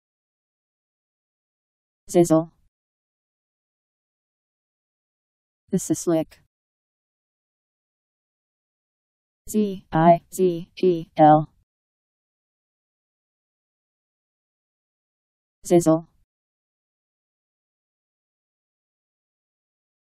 Zizel Meaning
Video shows what zizel means. The suslik..  Zizel Meaning. How to pronounce, definition audio dictionary. How to say zizel. Powered by MaryTTS, Wiktionary